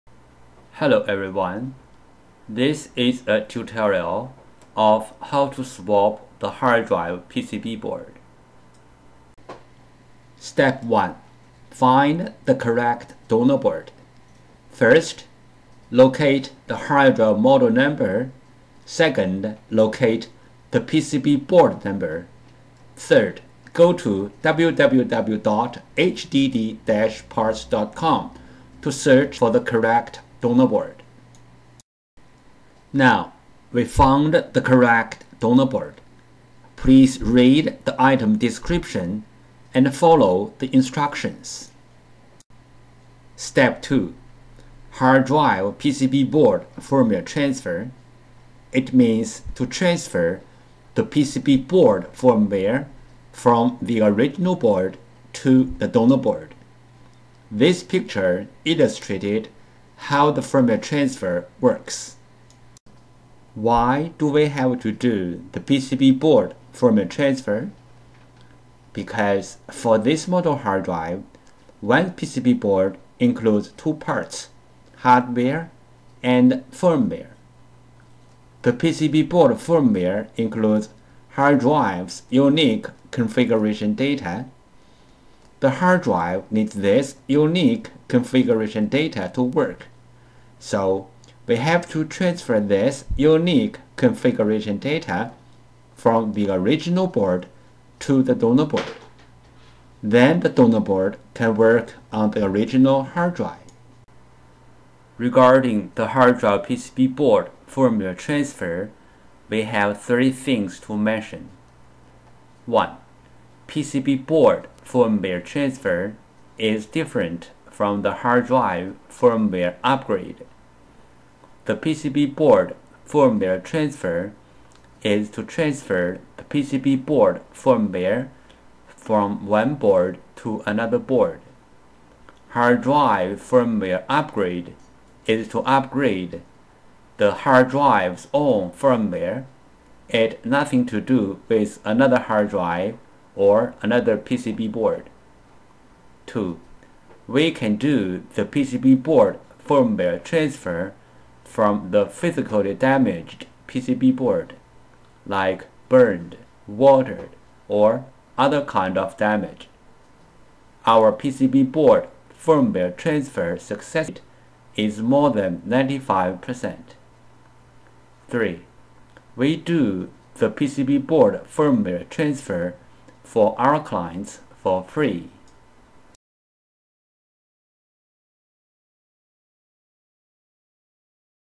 Seagate repair tip 100535704 ST250DM000 ST250DM000 ST3250318AS ST3320418AS ST3500312CS ST3500413AS S
This tutorial section is going to answer the questions above for each type hard drive PCB board.

Tutorial: how to fix Seagate hard drive by swap the PCB board
1. How to find the correct donor board
2. Why the PCB board firmware (BIOS) transfer is a MUST?
3. What is the PCB board firmware (BIOS)?
4. Where is the PCB board firmware (BIOS)?
5. How to transfer the hard drive PCB board firmware (BIOS)?